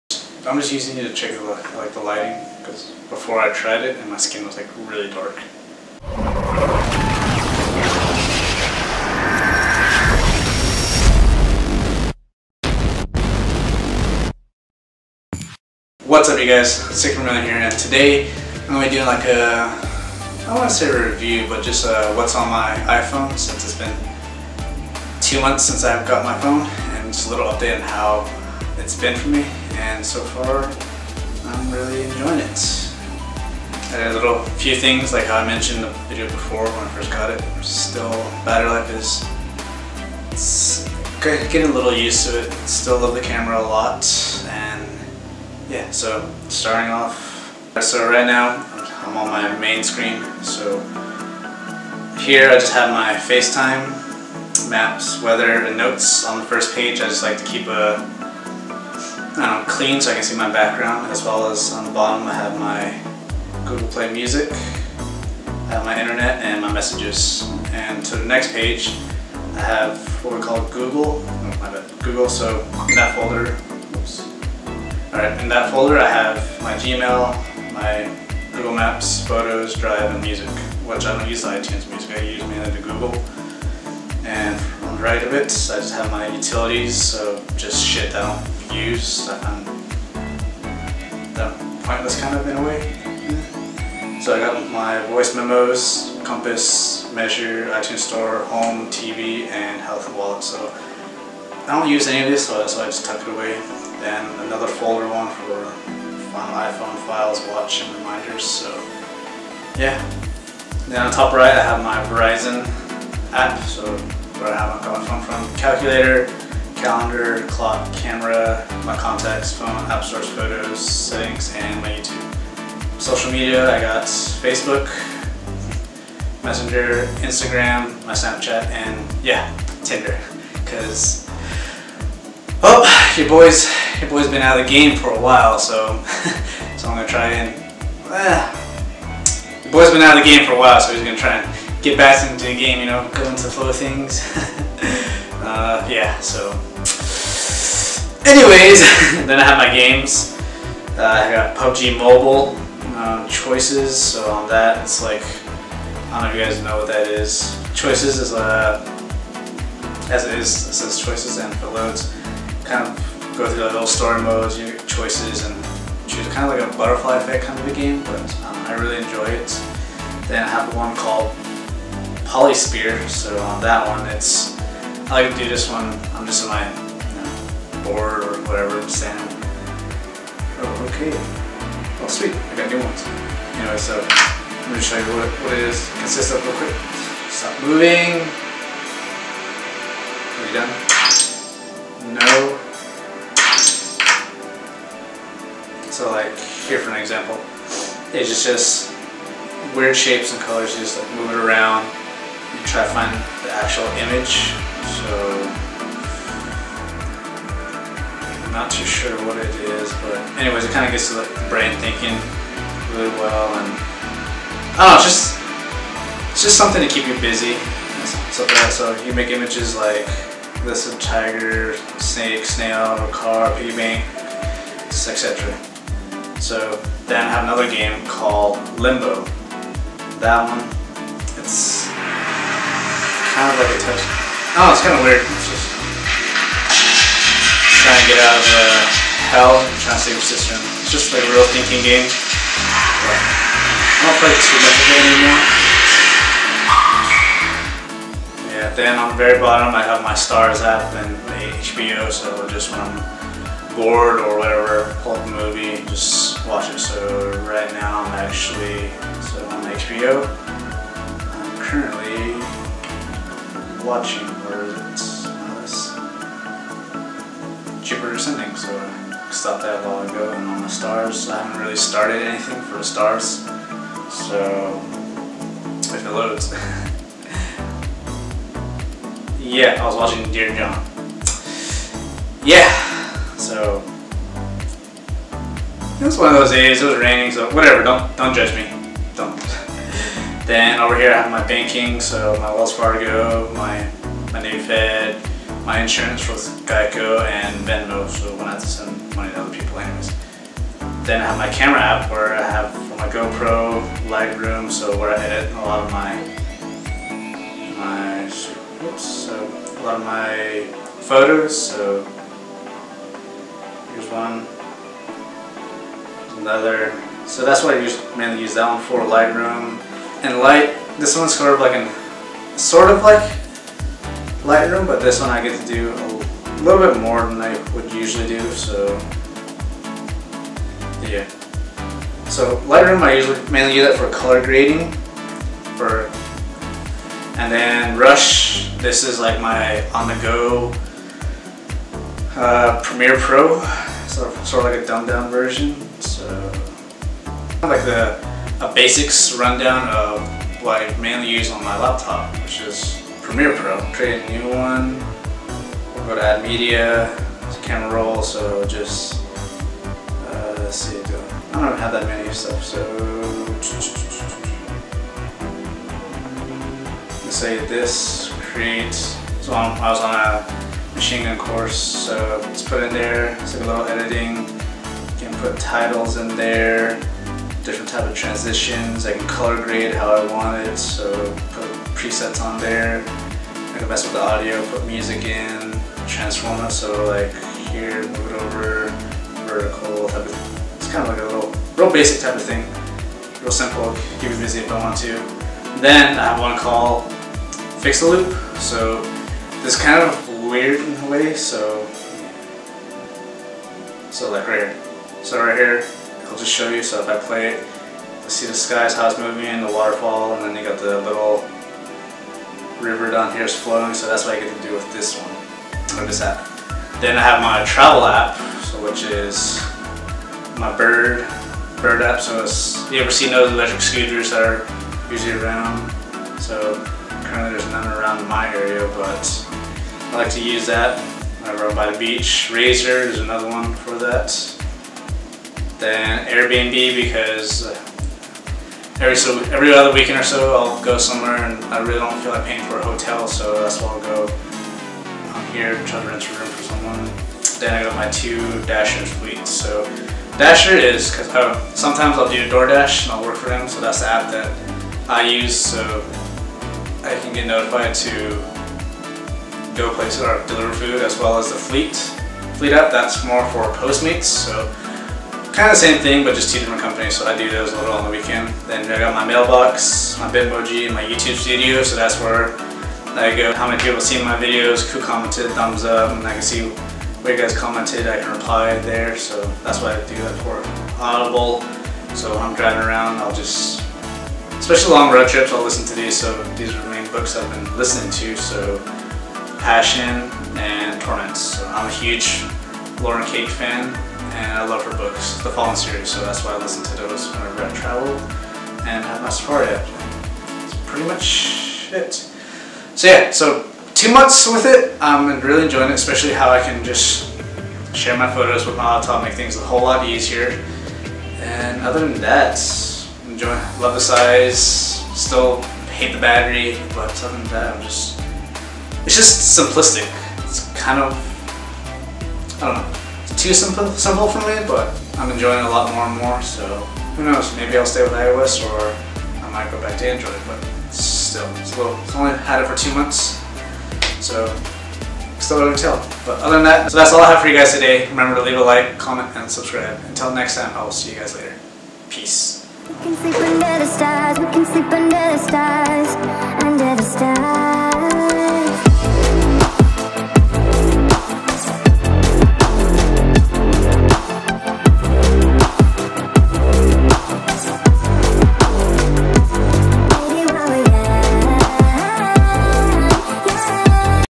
What's On My IPhone XS Max
What's Up Guys!! Here's a video of what's on my iPhone XS Max and what my thoughts about the iPhone after having it for 2 months now.

Leave a Like or Comment if you Enjoyed!!